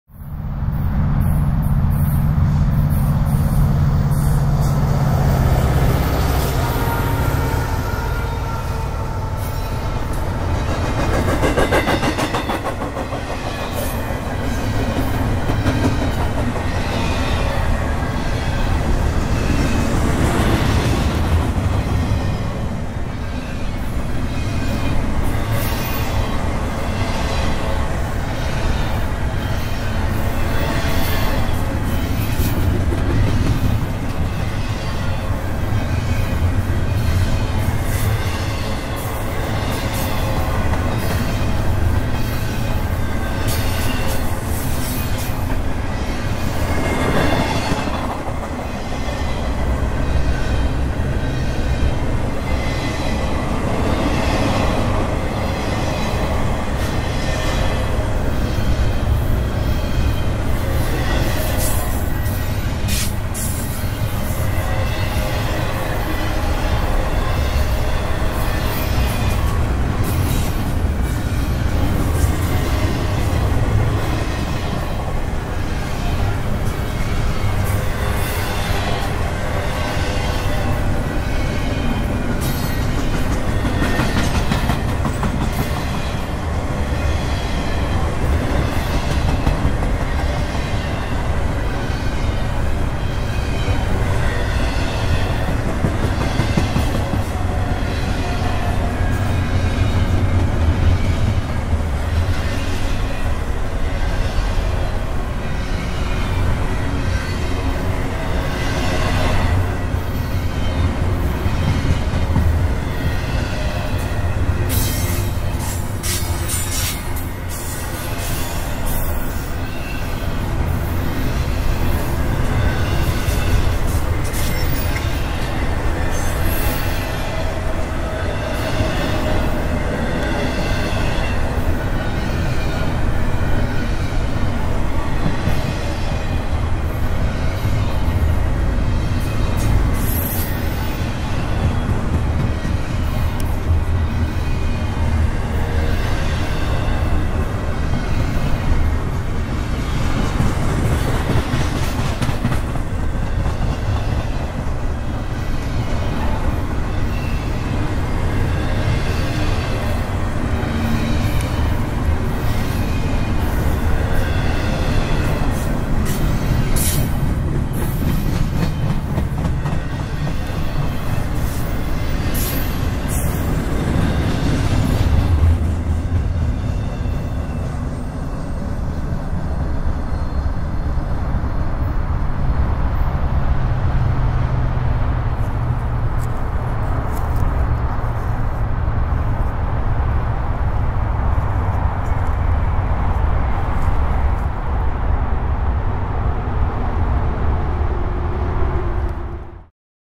NS 202 with SD70ACC 1840 & ET44AC 3648
Norfolk Southern 202 is eastbound in Huntsville, Alabama near Fat Harry's.

The locomotives are NS 1840 and NS 3648. SD70ACC DC to AC Conversion 1840 was built from NS SD70 2518.